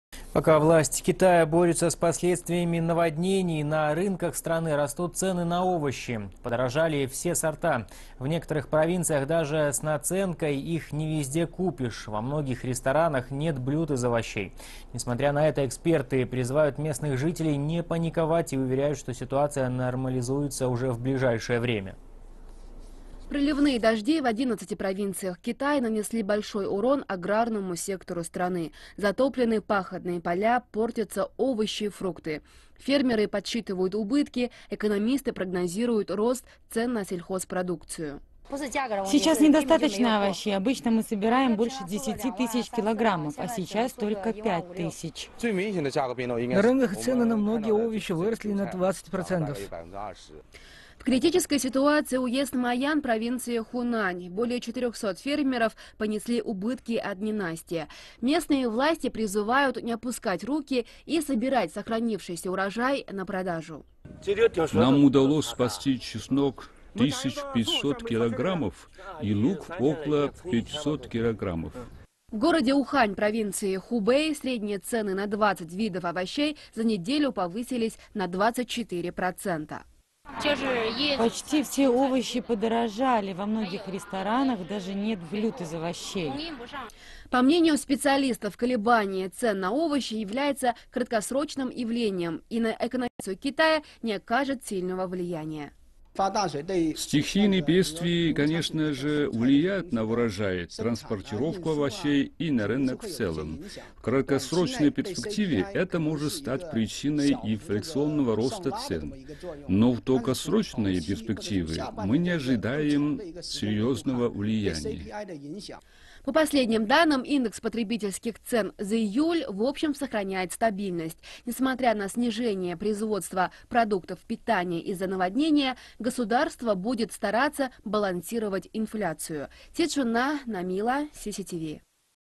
Из-за наводнений в Китае растут цены на овощи
Борьба со стихией